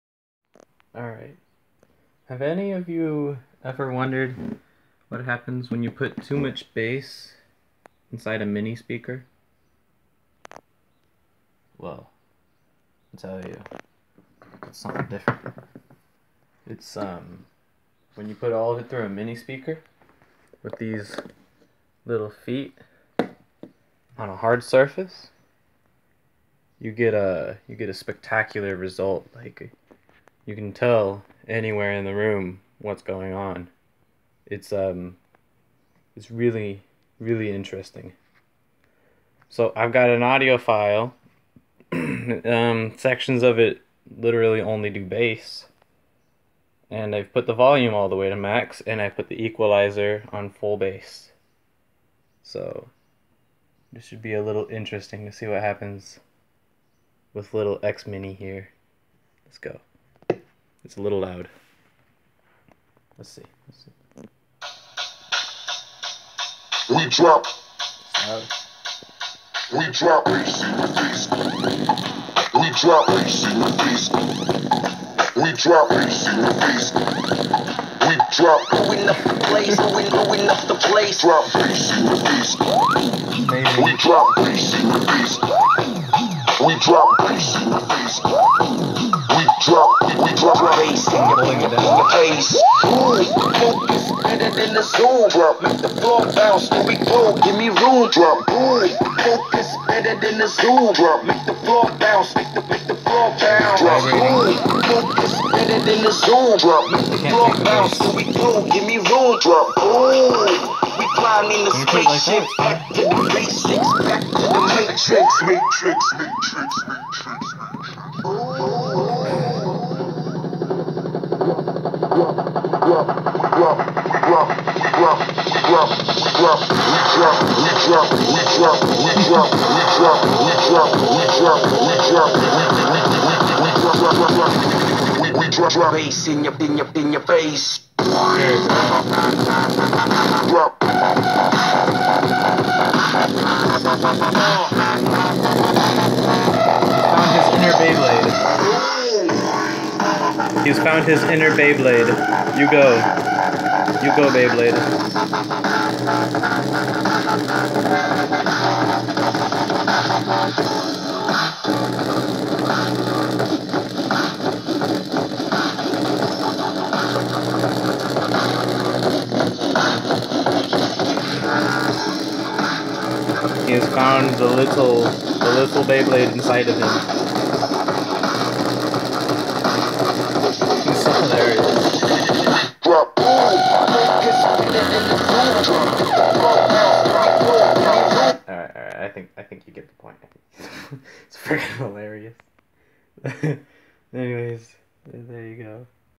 Too much bass in Minispeaker
Today class, we learn what happens when you put too much bass cannon through a six year old Minispeaker. Simply shocking!